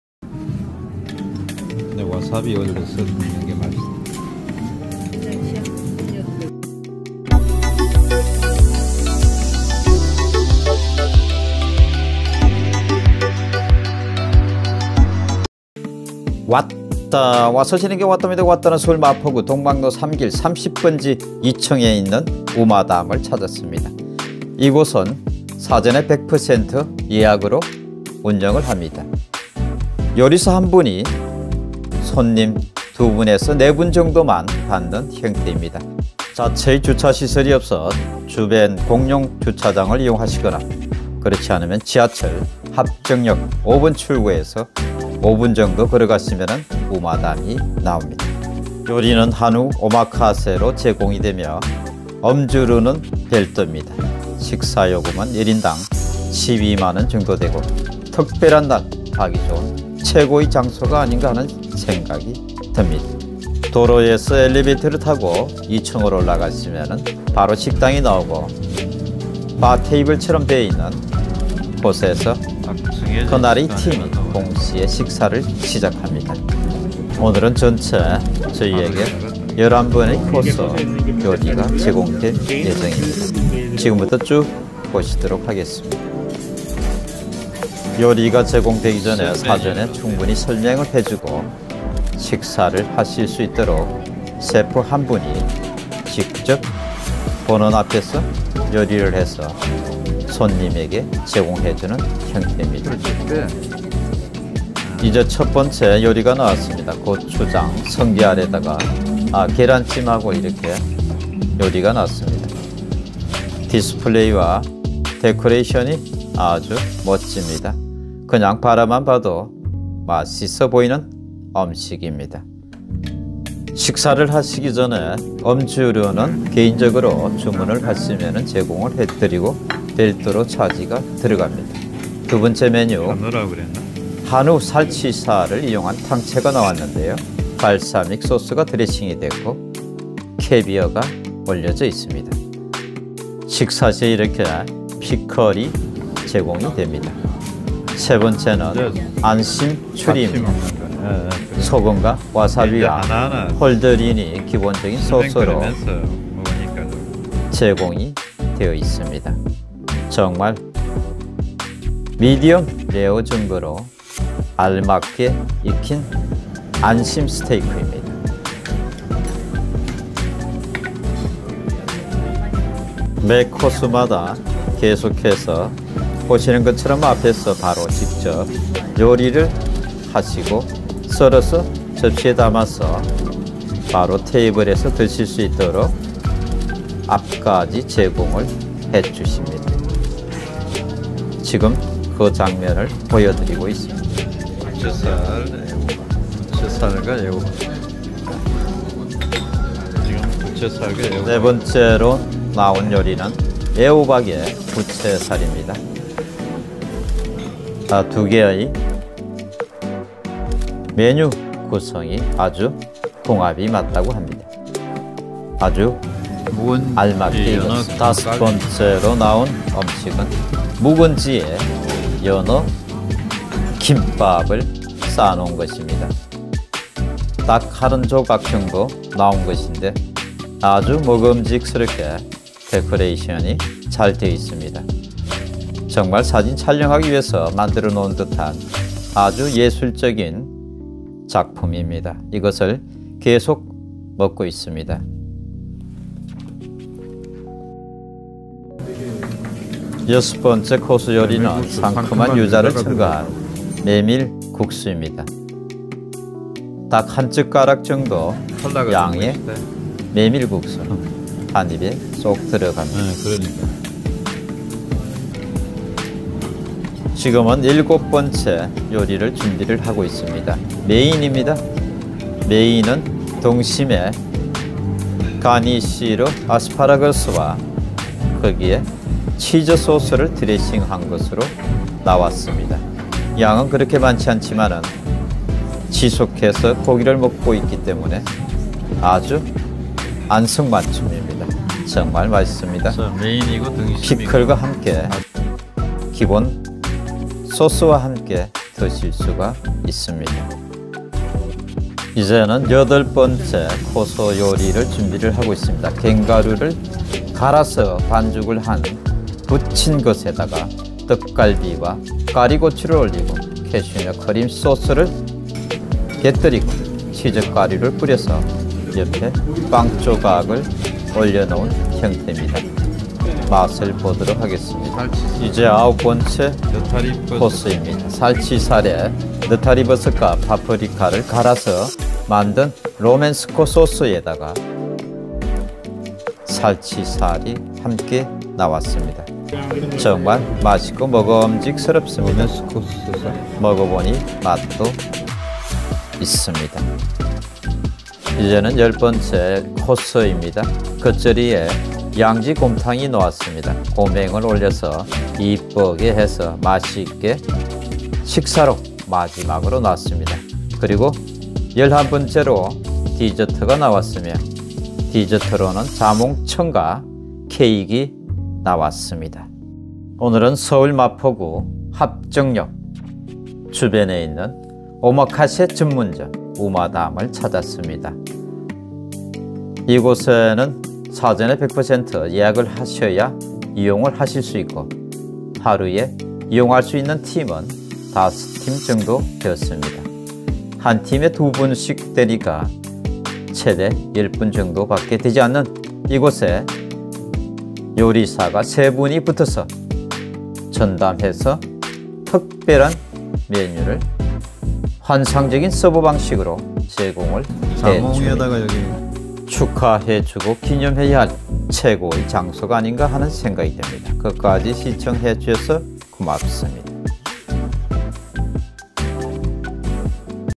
[맛집 ~ 오마카세]한우 오마카세에서 기억에 남는 특별한 순간을 만들어 주는 합정역 우마담 오마카세 내돈 주고 맛나게 잘 먹은 날,Korean beef Omakase
서울 마포구 독막로 3길 30번 2층에 있는 우마담은 한우 오마카세 전문점이다
한우로 구성된 메뉴이지만 오마카세 특성상 요리사와 손님이 하나가 되어 그날의 식사를 즐기는 독특한 분위기를 연출한다

이곳은 합정역 5번 출구로 나와서 걸어서 5분 정도의 거리에 있으며 자동차를 주차할 공간이 부족하여 주변에 있는 공용주차장에 주차하고 이곳을 찾아야 하는 불편함도 있다

특별한 날을 기념하기 위하여 한잔하는 분위기라면 차를 가지고 오지 않고 택시를 이용하거나 대중교통을 이용하는 편이 나을수가 있다

이곳은 사전 100% 예약제로 운영되며 오후 5시에 오픈하여 당일 예약손님은 동시간대에 식사를 하게 된다

물론 일행이 아니지만 동시간대에 예약을 받아서 한꺼번 식사가 이루어 지도록 하는 듯 하다

식사메뉴는 세트로 주방장 선정된 메뉴로 제공이 되며 음주류는 별도로 가산된다
필요한 음주류는 이곳에 도착하여 그때 주문 하시면 이용이 가능하다

특별한 날 기억을 남기기 위한 시간이라면 이곳을 추천한다
내돈주고 먹었지만 정말 지금도 기억에 남은 순간이다